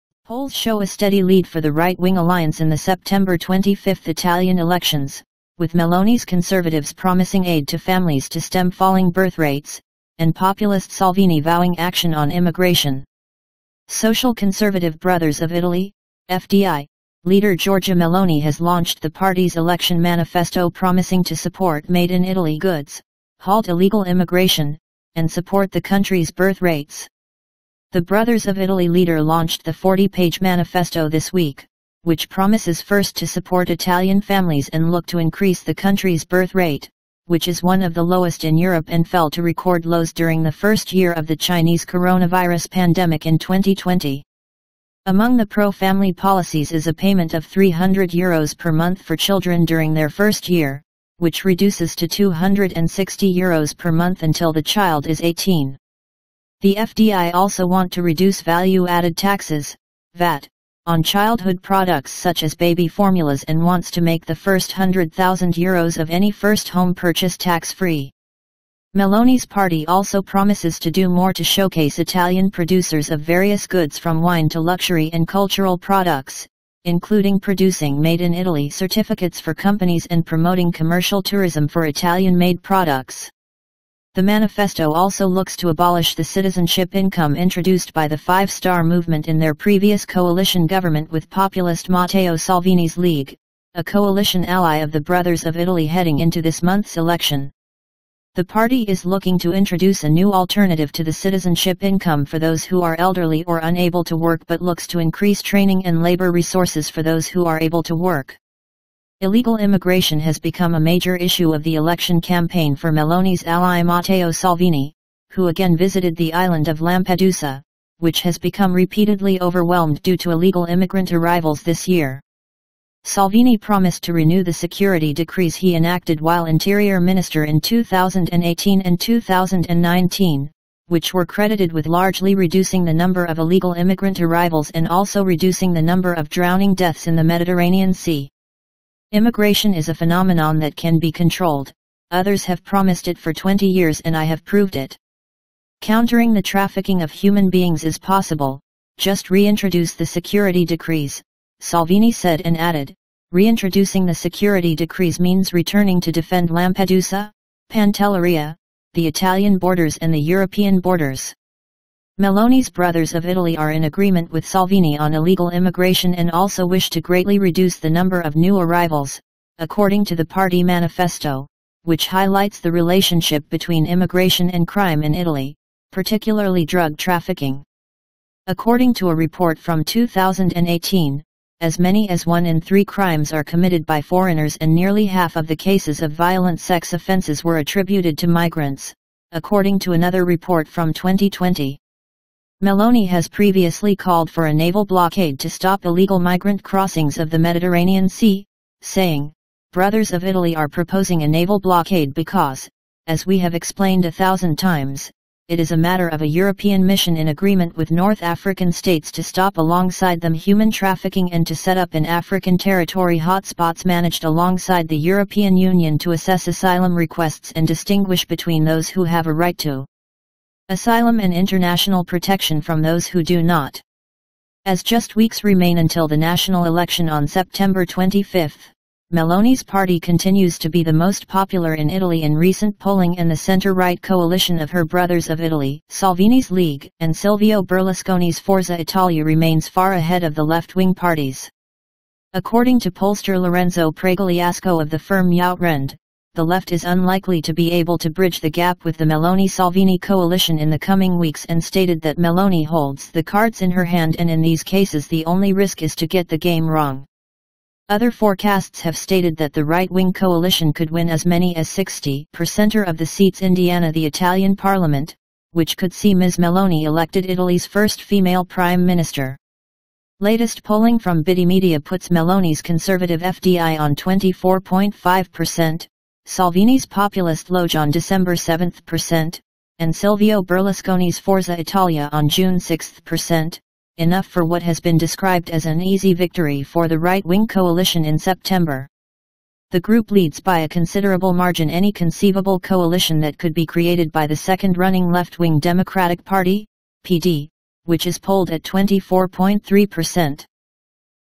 Meloni launches manifesto promising to halt illegal migration and supports Italian birth rates
Polls show a steady lead for the right-wing alliance in the September 25 Italian elections, with Meloni’s conservatives promising aid to families to stem falling birthrates, and populist Salvini vowing action on immigration.

Social conservative Brothers of Italy (FdI) leader Giorgia Meloni has launched the party’s election manifesto promising to support “Made in Italy” goods, halt illegal immigration, and support the country’s birth rates.

The Brothers of Italy leader launched the 40-page manifesto this week, which promises first to support Italian families and look to increase the country’s birth rate, which is one of the lowest in Europe and fell to record lows during the first year of the Chinese coronavirus pandemic in 2020.

Among the pro-family policies is a payment of 300 euros per month for children during their first year, which reduces to 260 euros per month until the child is 18. The FdI also want to reduce value-added taxes (VAT) on childhood products such as baby formulas and wants to make the first hundred thousand euros of any first home purchase tax-free.

Meloni’s party also promises to do more to showcase Italian producers of various goods from wine to luxury and cultural products, including producing “Made in Italy” certificates for companies and promoting commercial tourism for Italian-made products.

The manifesto also looks to abolish the citizenship income introduced by the Five Star Movement in their previous coalition government with populist Matteo Salvini’s League, a coalition ally of the Brothers of Italy heading into this month’s election. The party is looking to introduce a new alternative to the citizenship income for those who are elderly or unable to work but looks to increase training and labour resources for those who are able to work.

Illegal immigration has become a major issue of the election campaign for Meloni’s ally Matteo Salvini, who again visited the island of Lampedusa, which has become repeatedly overwhelmed due to illegal immigrant arrivals this year.

Salvini promised to renew the security decrees he enacted while Interior Minister in 2018 and 2019, which were credited with largely reducing the number of illegal immigrant arrivals and also reducing the number of drowning deaths in the Mediterranean Sea.

“Immigration is a phenomenon that can be controlled, others have promised it for twenty years and I have proved it. Countering the trafficking of human beings is possible, just reintroduce the security decrees,” Salvini said and added, “Reintroducing the security decrees means returning to defend Lampedusa, Pantelleria, the Italian borders and the European borders.”

Meloni’s Brothers of Italy are in agreement with Salvini on illegal immigration and also wish to greatly reduce the number of new arrivals, according to the party manifesto, which highlights the relationship between immigration and crime in Italy, particularly drug trafficking.

According to a report from 2018, as many as one in three crimes are committed by foreigners and nearly half of the cases of violent sex offences were attributed to migrants, according to another report from 2020.

Meloni has previously called for a naval blockade to stop illegal migrant crossings of the Mediterranean Sea, saying, “Brothers of Italy are proposing a naval blockade because, as we have explained a thousand times, it is a matter of a European mission in agreement with North African states to stop — alongside them — human trafficking and to set up in African territory hotspots managed alongside the European Union to assess asylum requests and distinguish between those who have a right to asylum and international protection from those who do not.”

As just weeks remain until the national election on September 25th, Meloni’s party continues to be the most popular in Italy in recent polling and the centre-right coalition of her Brothers of Italy, Salvini’s League and Silvio Berlusconi’s Forza Italia remains far ahead of the left-wing parties.

According to pollster Lorenzo Pregliasco of the firm Youtrend, the left is unlikely to be able to bridge the gap with the Meloni-Salvini coalition in the coming weeks and stated that Meloni holds “the cards in her hand and in these cases the only risk is to get the game wrong.”

Other forecasts have stated that the right-wing coalition could win as many as 60 per cent of the seats in the Italian parliament, which could see Ms Meloni elected Italy’s first female Prime Minister.

Latest polling from BidiMedia puts Meloni’s conservative Fdi on 24.5 per cent, Salvini’s populist Leauge on 12.7 per cent, and Silvio Berlusconi’s Forza Italia on 6.6 per cent, enough for what has been described as an “easy victory” for the right-wing coalition in September. The group leads by a considerable margin any conceivable coalition that could be created by the second-running left-wing Democratic Party (PD), which is polled at 24.3 per cent.